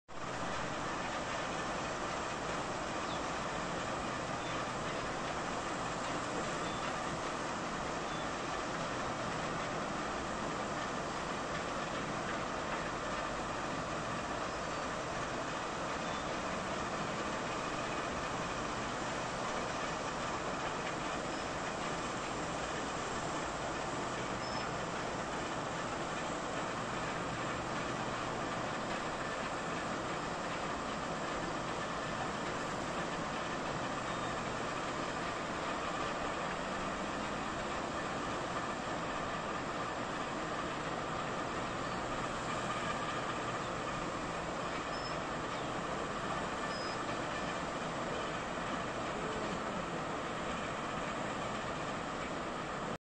loading di jetty Pt. Mandiri Inti Perkasa
tongkang kapal Prima loading batubara